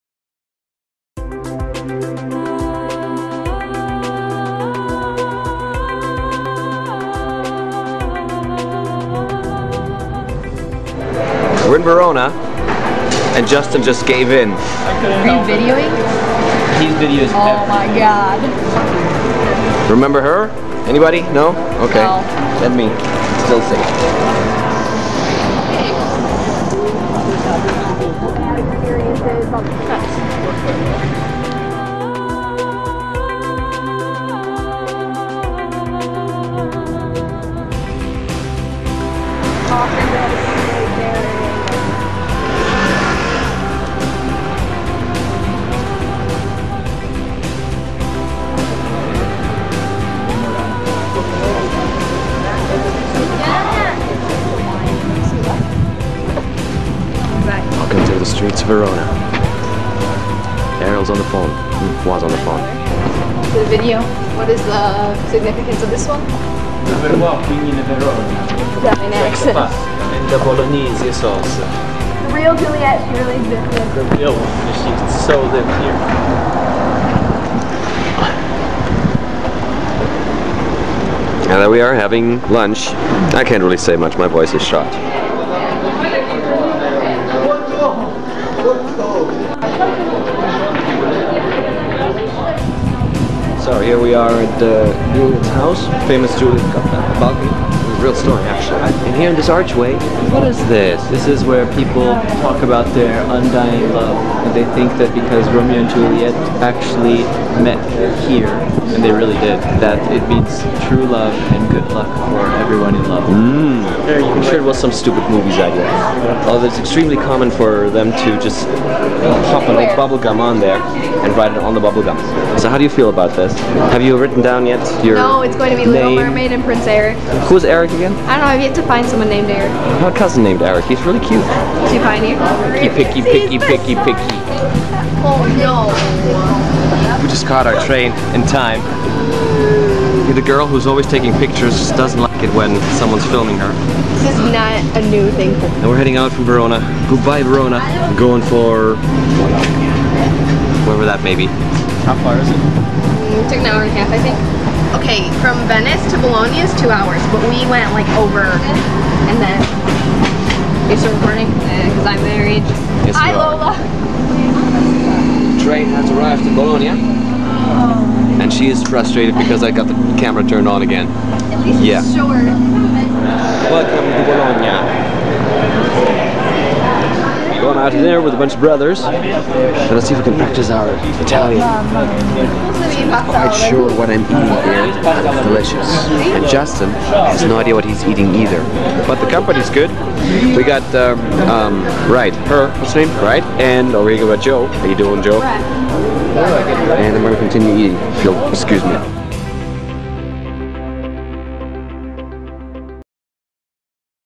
VideoBlog: Invasion: Italy! -- Letters to Romeo
We hit up Verona and meet my old friend Arielle. We also got to see the location where Romeo and Juliette lived. You know, the real ones. 'cause they really existed...